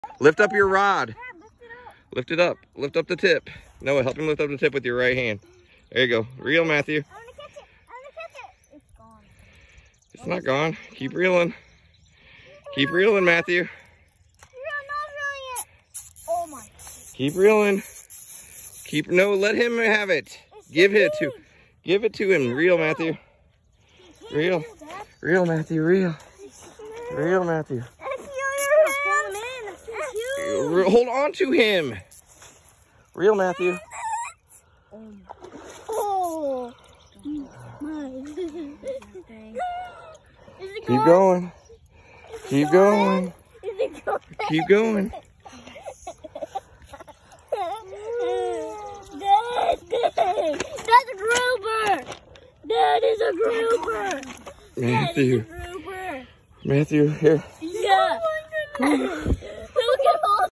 5 yr old reeling in 4.2 pound bass
What a memory